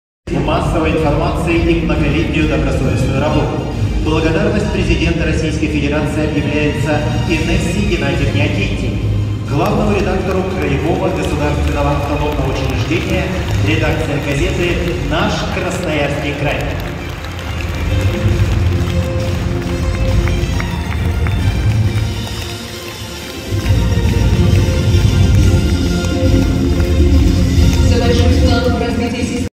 Награждение И. Г. Акентьевой
Главный редактор НКК получила благодарность президента